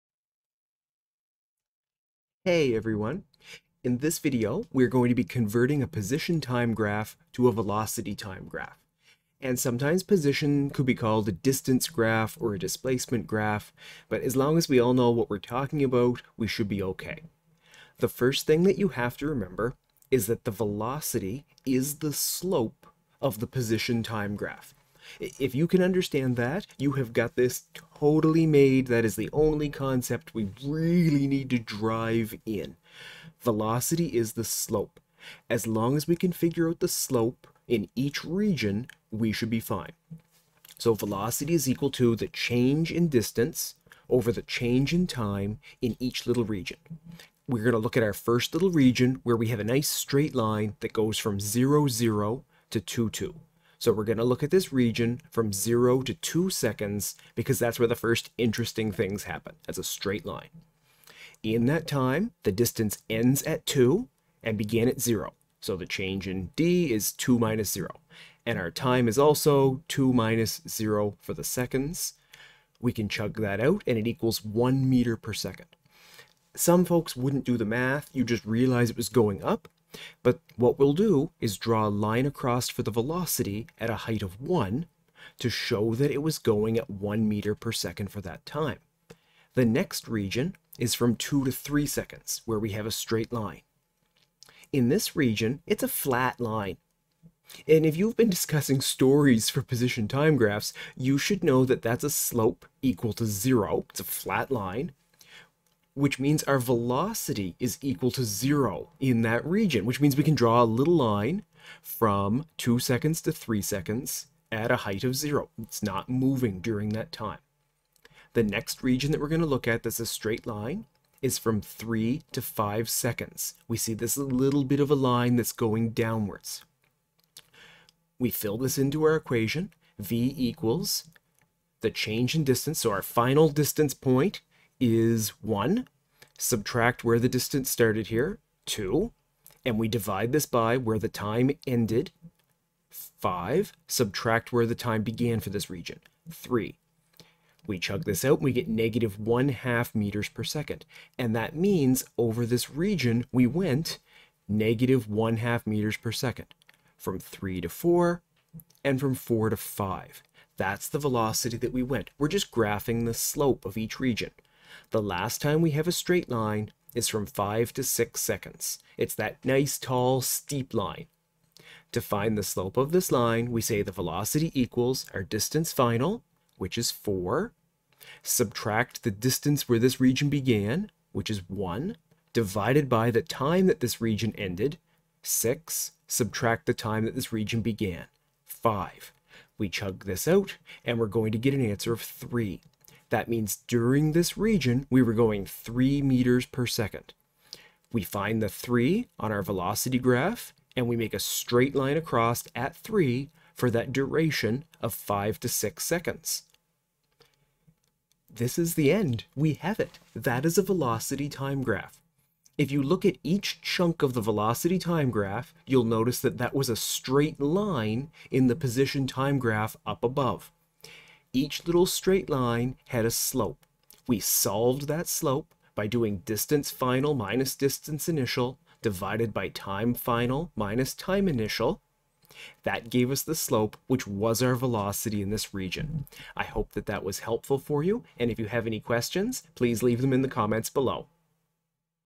How To Convert a Position/Time Graph to a Velocity/Time Graph
For each region of the position/time graph use the equation  v = (d2 - d1)/(t2-t1)  This will give you the slope of that region, which is what we will graph in our velocity time graph.

This example problem has 4 regions with separate slopes.  We will calculate each of the four slopes and then use them to graph the velocity/time graph below.